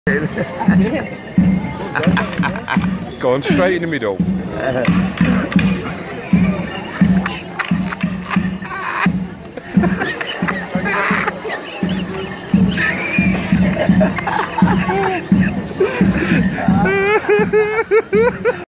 Funniest Morris Dancing
best video ever, drunk Jamie inviting himself to Morris dance